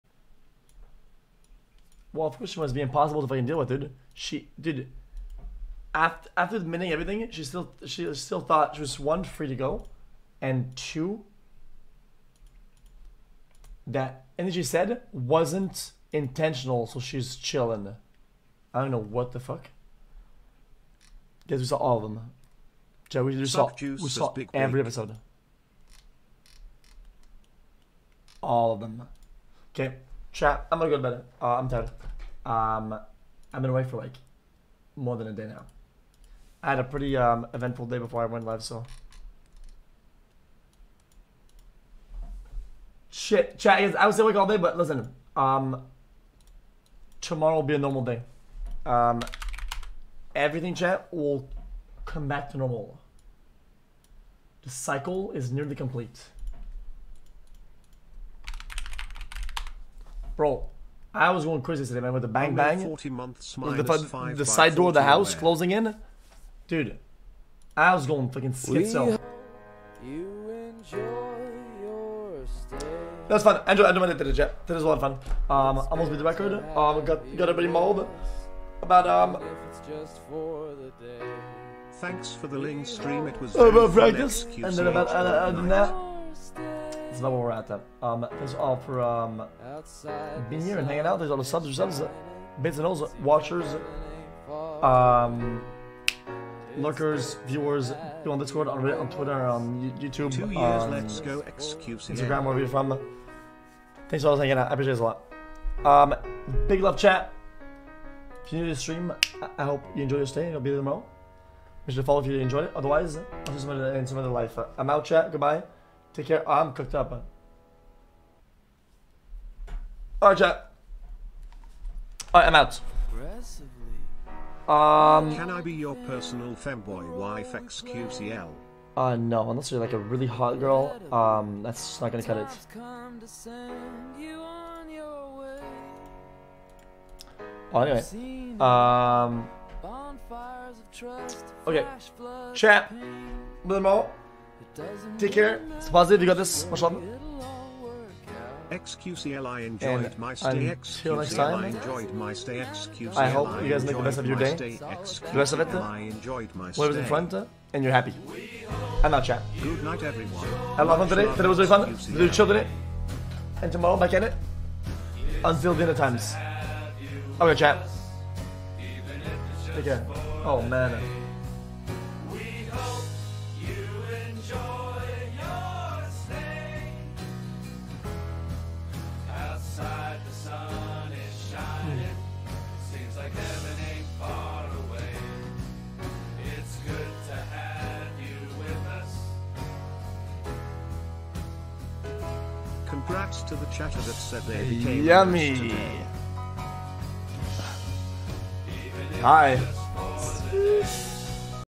XQC ends stream after jcs SO CUTE BUT THE FINGER SKIN PICKIN GOTTA END
GG yall love ya bud xqc is the orginal content
Not monetized content Stay safe yall!¬